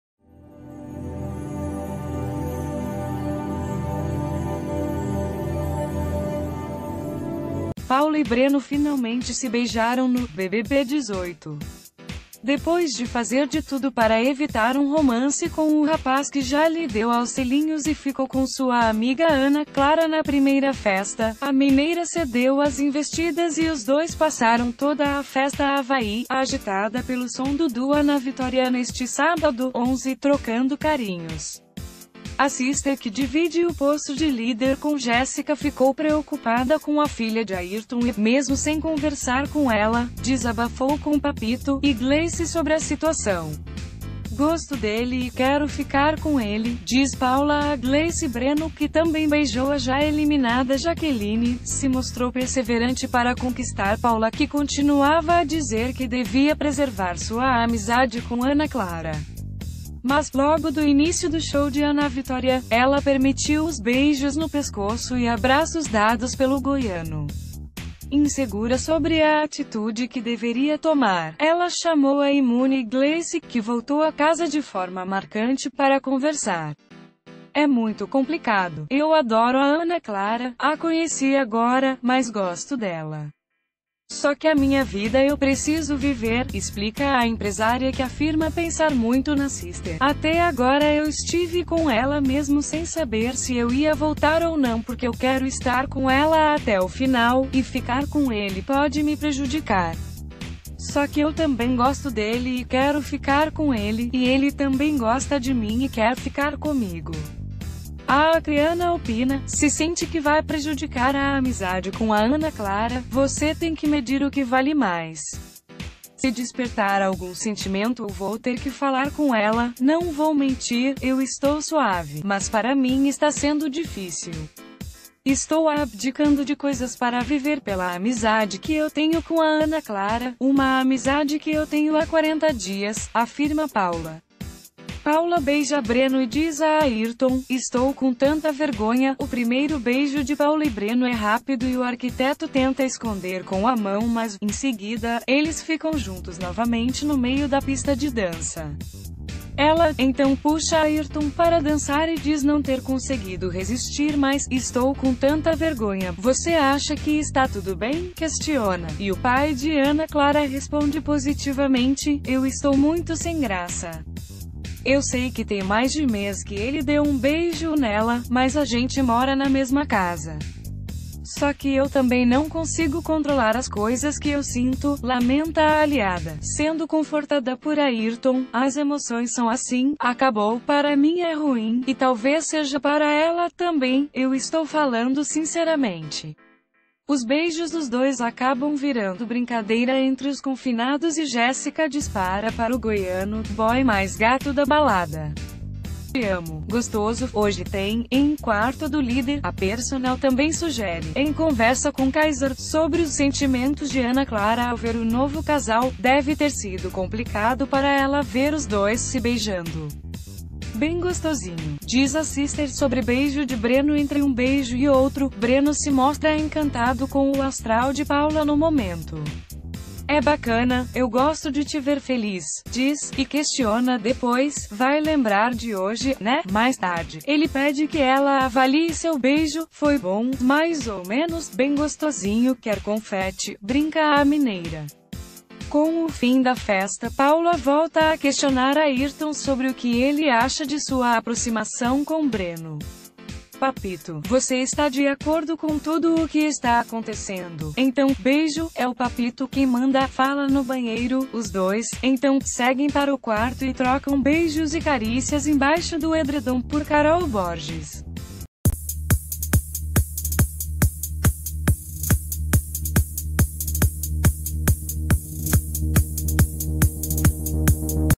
'BBB18': Paula e Breno se beijam em festa e dormem juntos. 'Bem gostosinho'| BBB18 Notícias 24 ho...
'BBB18': Paula e Breno se beijam em festa e dormem juntos. 'Bem gostosinho'| BBB18 Notícias 24 horas ao vivo
Paula e Breno finalmente se beijaram no "BBB18". Depois de fazer de tudo para evitar um romance com ...